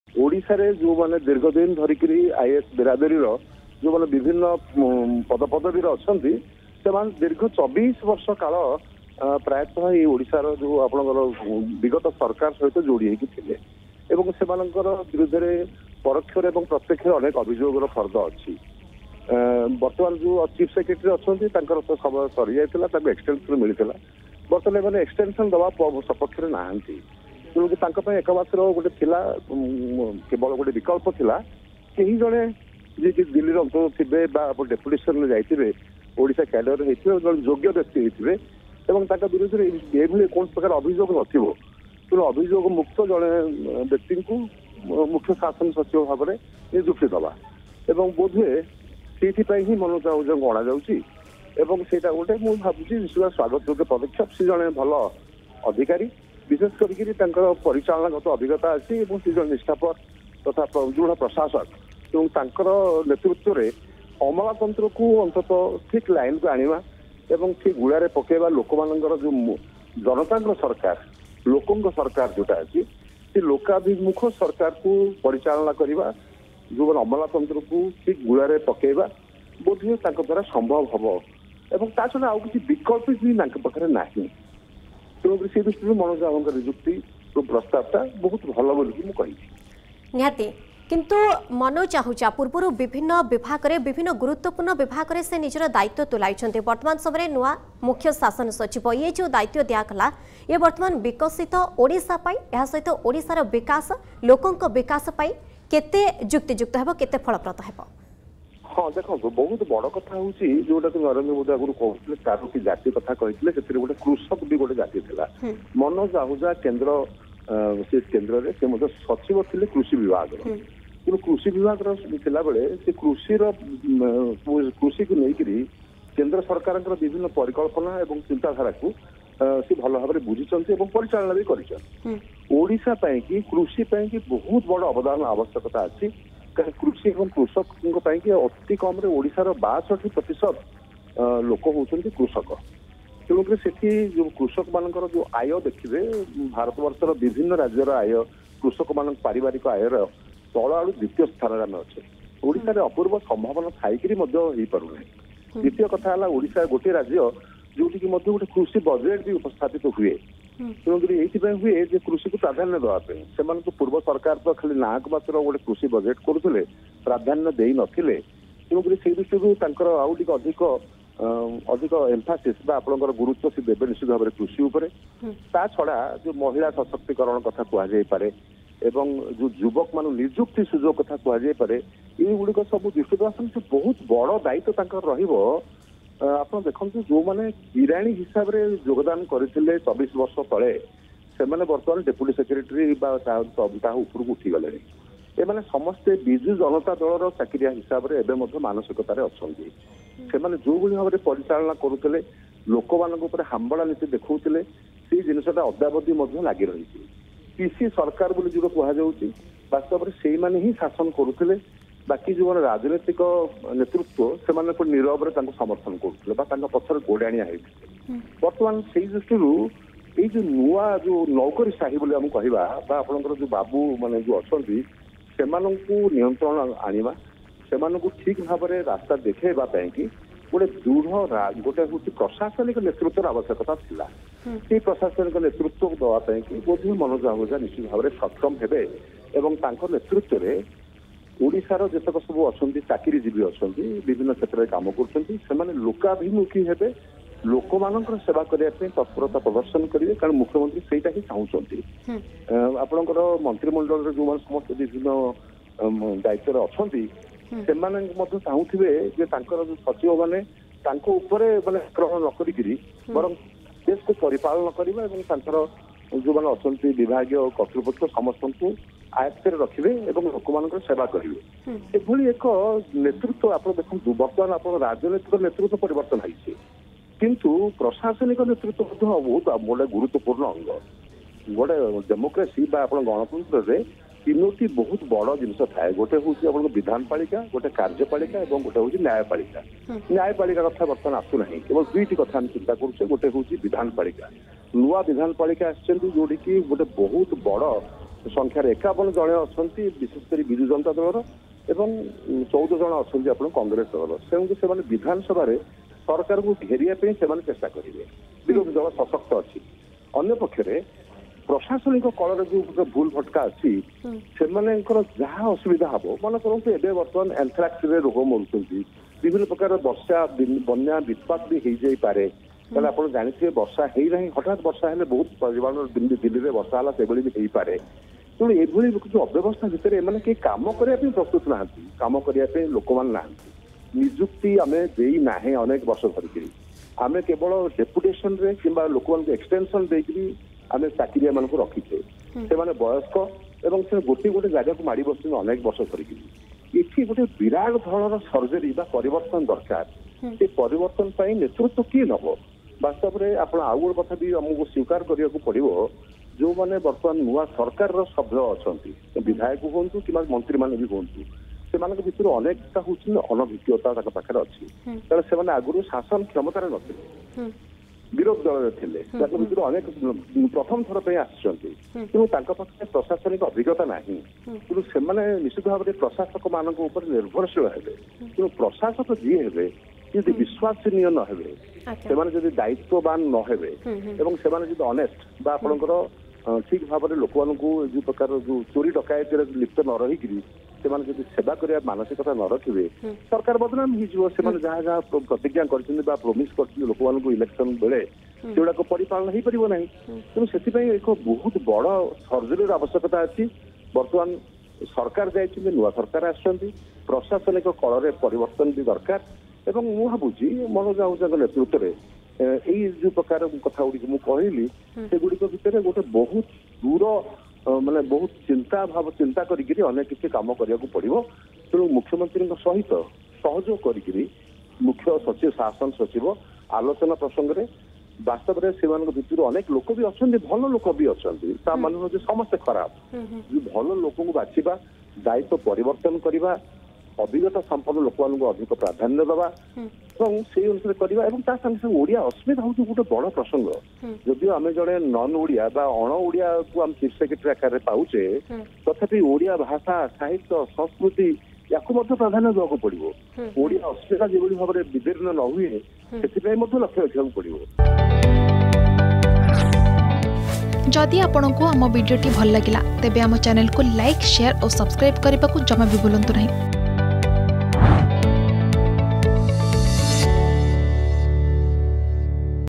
ମନୋଜ ଆହୁଜା ହେଲେ ମୁଖ୍ୟ ଶାସନ ସଚିବ || Chief Secretary Manoj Ahuja
ମନୋଜ ଆହୁଜା ହେଲେ ମୁଖ୍ୟ ଶାସନ ସଚିବ । 1990 ବ୍ୟାଚର ଆଇଏଏସ ଅଧିକାରୀ ହେଉଛନ୍ତି ମନୋଜ ଆହୁଜା ।
ଗତ ମଙ୍ଗଳବାର ମନୋଜଙ୍କୁ ରିଲିଫ କରିଥିଲା କେନ୍ଦ୍ର କ୍ୟାବିନେଟ ।